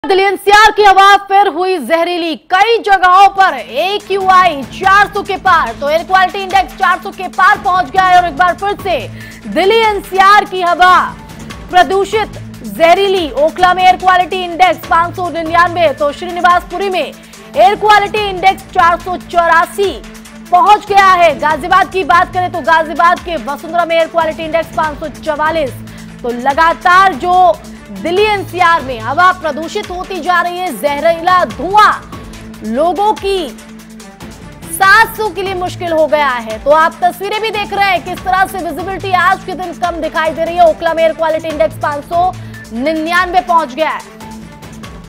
Delhi-NCR की हवा फिर हुई जहरीली
दिल्ली-एनसीआर की हवा फिर हुई जहरीली…कई जगहों पर AQI 400 के पार…ओखला में एयर क्वालिटी इंडेक्स 599…श्रीनिवासपुरी में AQI 484….गाजियाबाद के वसुंधरा में AQI 544